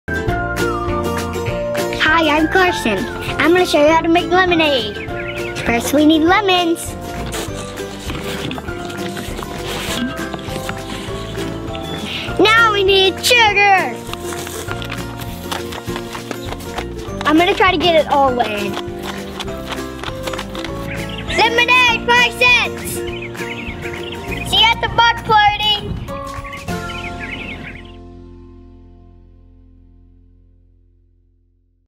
How to Make Lemonade (by Carson)
Carson shows us how to make lemonade for the Grand Opening of Kincade Homes. We're planning a block party, and you're invited.